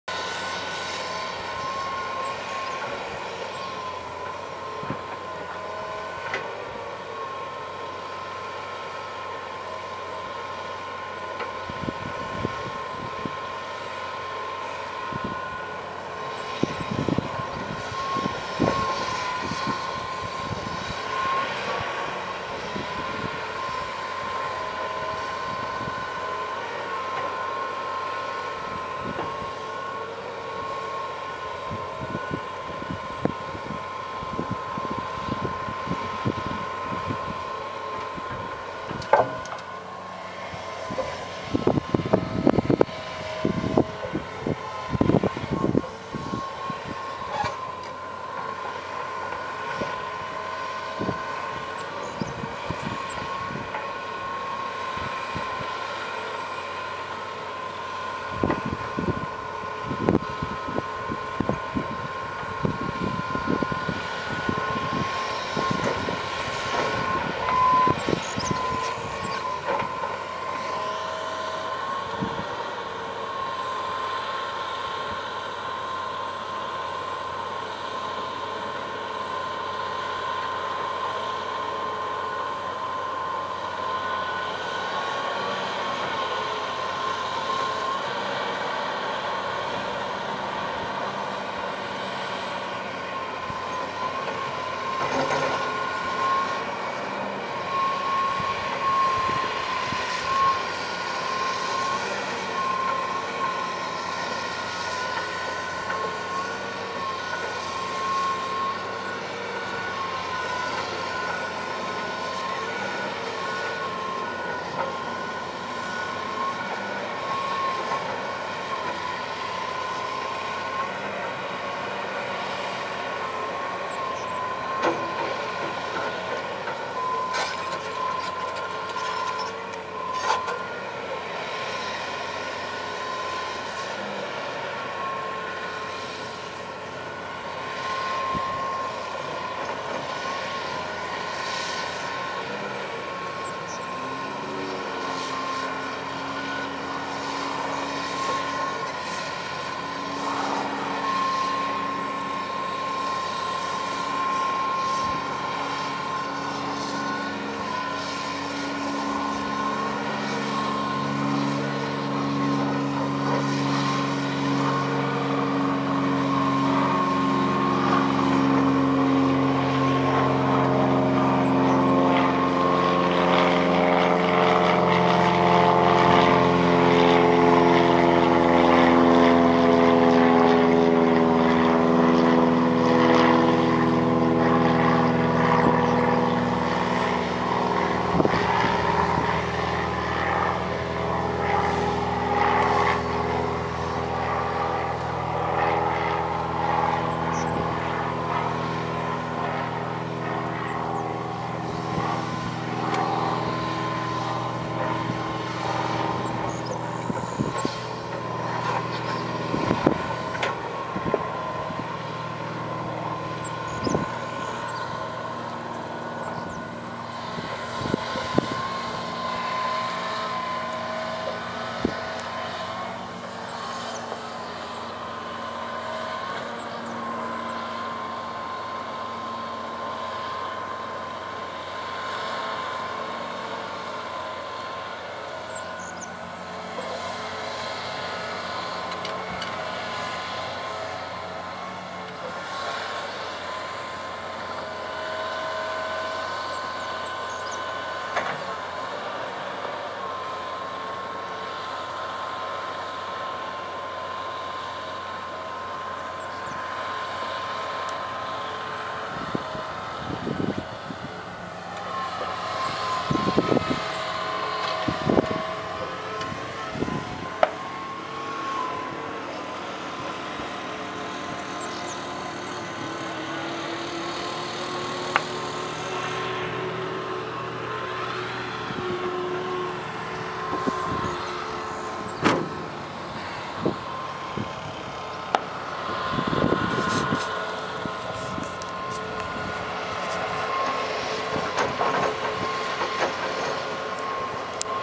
John Deere Excavator Working on Dike
A John Deere 230C LC excavator is working on a dike.